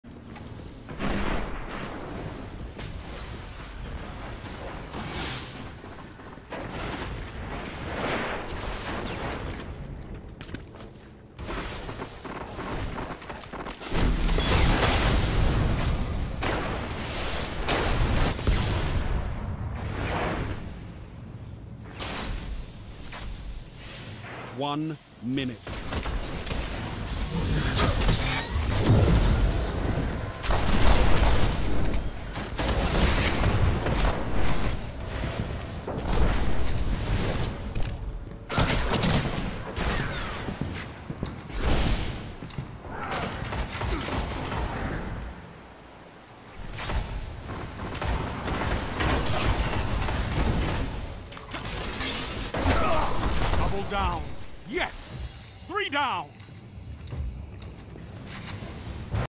Messed up blade dance; came back and got triple kill
Uploaded by: Thick n Furious